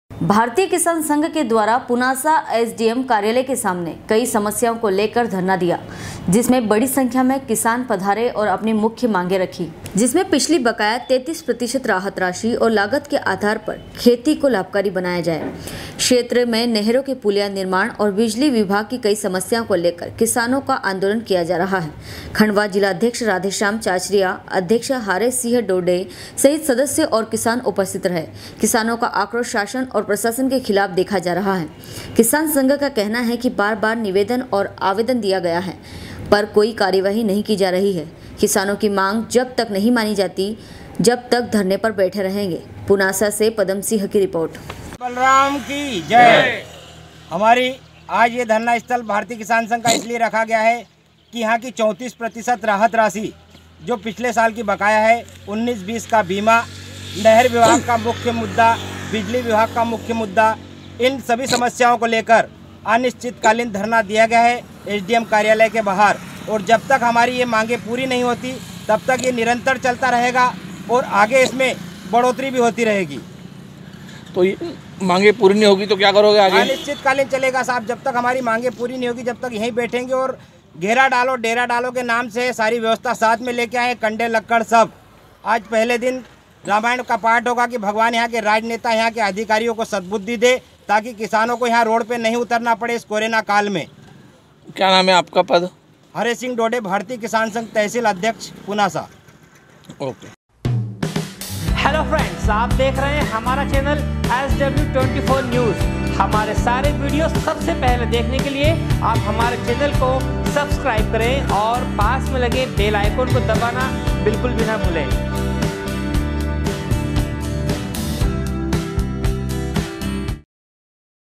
SW 24 News Madhyapradesh
भारतीय किसान संघ के द्वारा पुनासा ैक्ड कार्यालय के सामने कई समस्याओं को लेकर धरना दिया जिसमें बडी संख्या मे किसान पधारे   और अपनी मुख्य मांगे रखी। पुनासा से पदमसिंह की रिपोर्ट